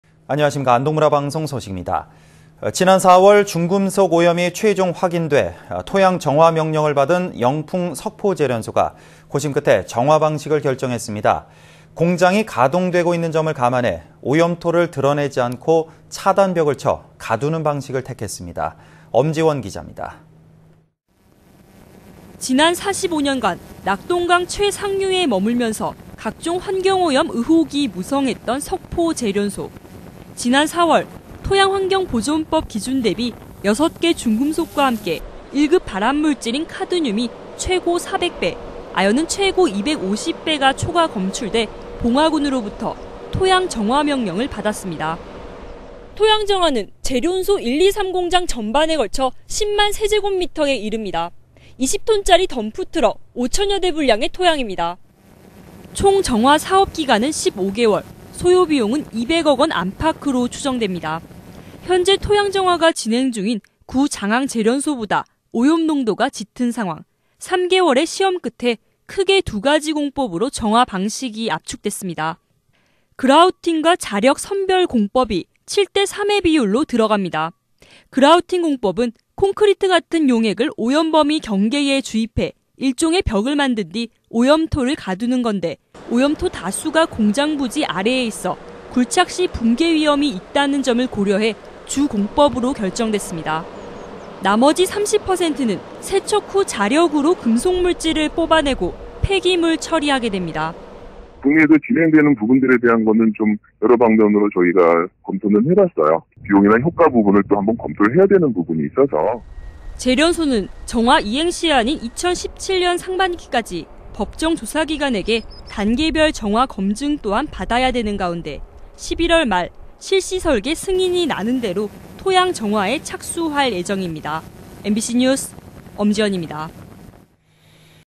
[안동MBC뉴스]R]석포제련소 토양정화 시작
2015/10/13 18:20:28    작성자 : 엄지원

◀ＡＮＣ▶
지난 4월 중금속 오염이 최종 확인돼
토양정화명령을 받은 영풍 석포제련소가,
고심끝에 정화방식을 결정했습니다.

공장이 가동되고 있는 점을 감안해
오염토를 드러내지 않고
차단벽을 쳐 가두는 방식을 택했습니다.

엄지원 기자
◀ＥＮＤ▶
◀ＶＣＲ▶

지난 45년간 낙동강 최상류에 머물면서
각종 환경오염 의혹이 무성했던 석포제련소.

지난 4월 토양환경보전법 기준 대비
6개 중금속과 함께, 1급 발암물질인 카드뮴이
최고 400배, 아연은 최고 250배가 초과 검출돼
봉화군으로부터 토양정화명령을 받았습니다.

(S/U)토양정화는 제련소 1·2·3공장 전반에 걸쳐 총 10만㎥에 이릅니다. 20톤급 덤프트럭
5천대 분량의 토양입니다.

총 정화사업 기간은 15개월,
소요비용은 200억원 안팎으로 추정됩니다.

현재 토양정화가 진행중인 (구)장항제련소보다
오염농도가 짙은 상황, 3개월의 시험 끝에
크게 2가지 공법으로 정화방식이 압축됐습니다.

'그라우팅'과 '자력선별' 공법이
7 대 3의 비율로 들어갑니다.

(C.G)그라우팅 공법은
콘크리트 같은 용액을 오염범위 경계에 주입해
일종의 벽을 만든 뒤 오염토를 가두는 건데(끝)

오염토 다수가 공장부지 아래에 있어
굴착시 붕괴위험이 있다는 점을 고려해
주공법으로 결정됐습니다.

나머지 30%는 세척후 자력으로
금속물질을 뽑아내고 폐기물 처리하게 됩니다.

◀ＳＹＮ▶환경보건기술연구원
국내에서 진행되는 (토양정화 사례) 부분은 여러 방법으로 검토해봤어요. 비용이나 효과를 검토해야 되는 부분이 있어서..

제련소는 정화 이행시한인 2017년 상반기까지
법정 조사기관에게 단계별 정화검증 또한
받아야 되는 가운데, 11월 말 실시설계 승인이
나는대로 토양정화에 착수할 예정입니다.